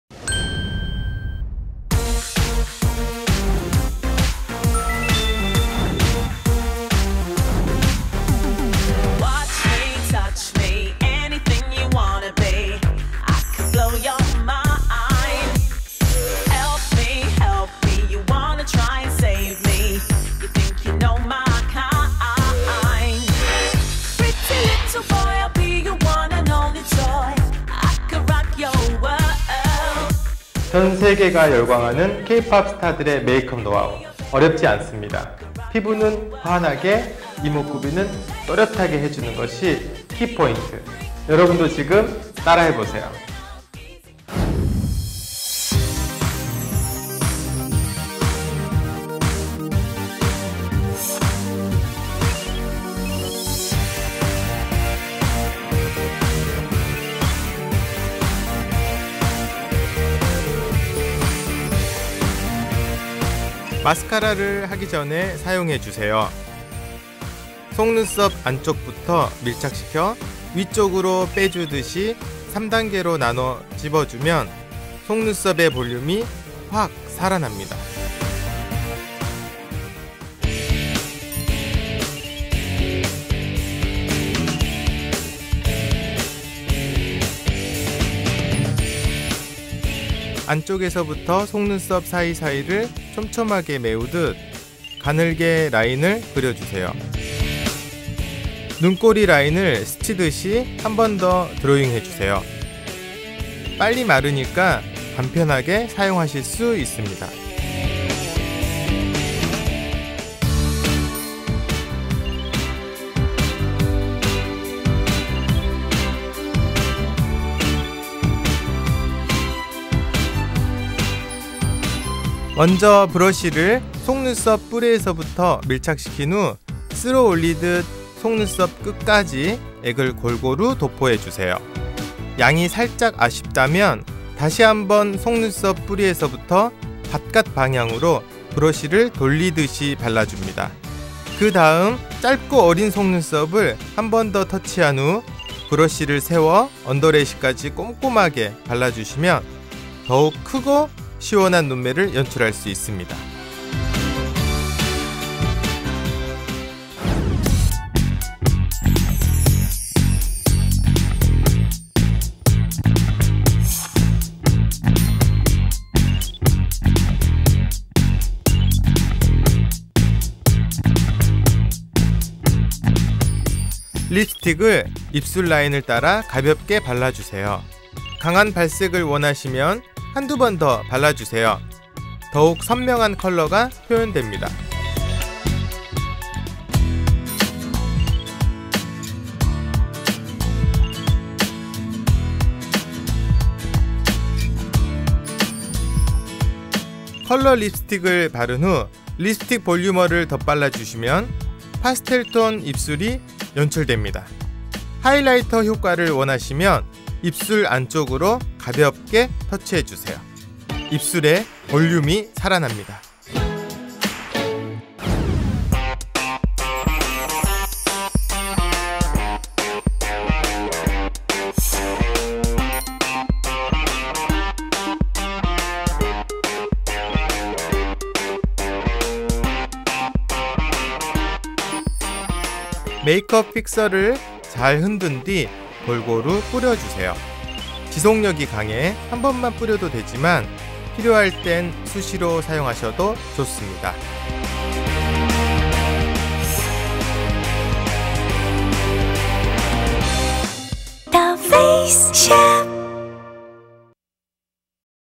더페이스샵 THEFACESHOP 손대식 메이크업 Lesson K-POP 메이크업
메이크업 아티스트 손대식의 노하우를 레슨하다!
메이크업 아티스트 손대식의 경험과 노하우를 고스란히 영상 공개!
그의 친절한 어드바이스를 '그대로' 받은 것 처럼 누구나 쉽고 빠르게 트렌디한 메이크업을 완성하게 도와 드립니다.